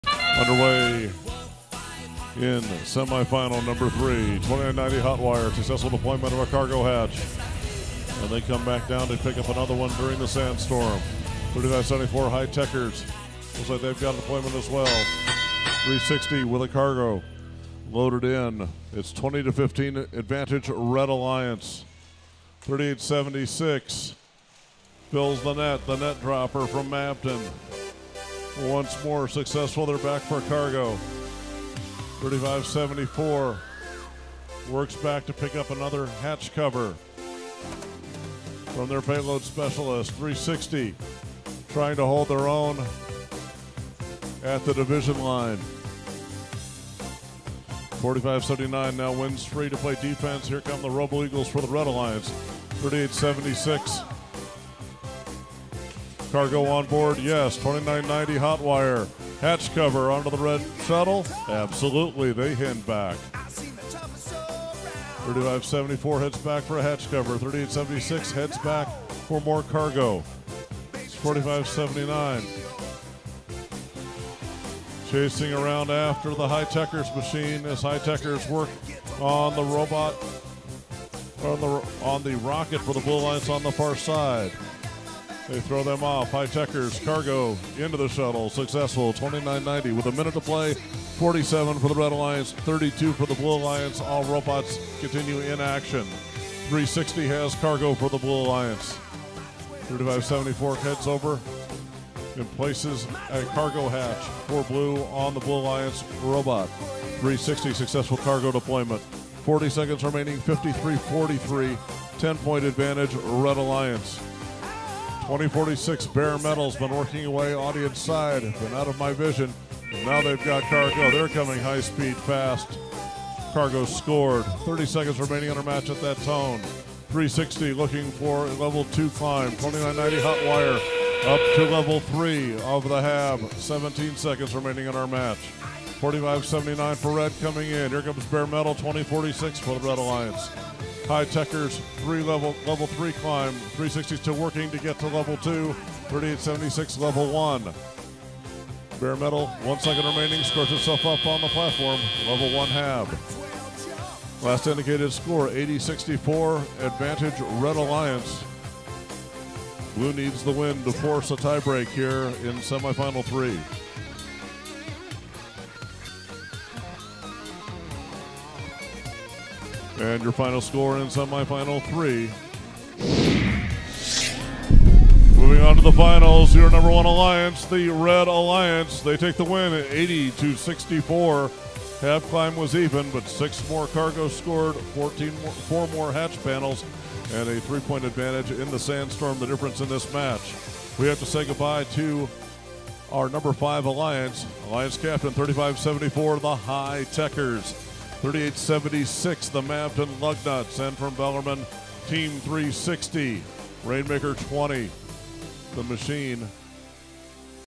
2019 PNW District Auburn Event WIDE Semifinal 1 Match 2
2019 FRC PNW District Auburn Event Week #5

Footage of the 2019 FRC PNW District Auburn Event is coutesy of the FIRST Washington A/V Crew